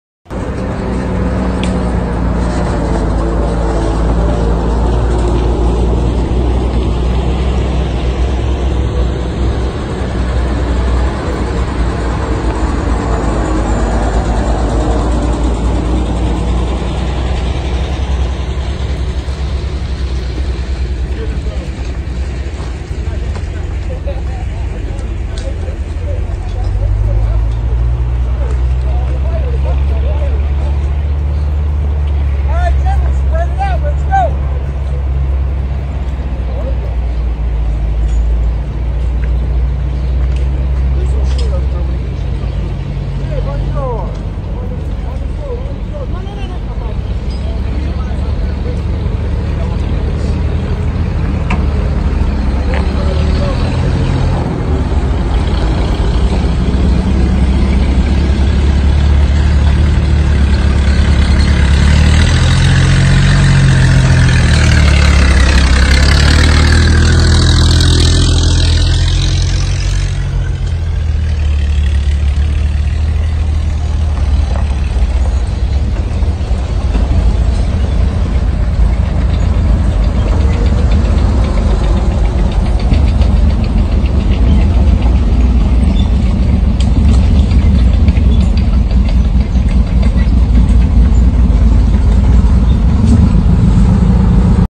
Field Operations
Some footage of our field events. We sure did miss these this year thanks to COVID. Bring on 2021 and let’s hit the ground running.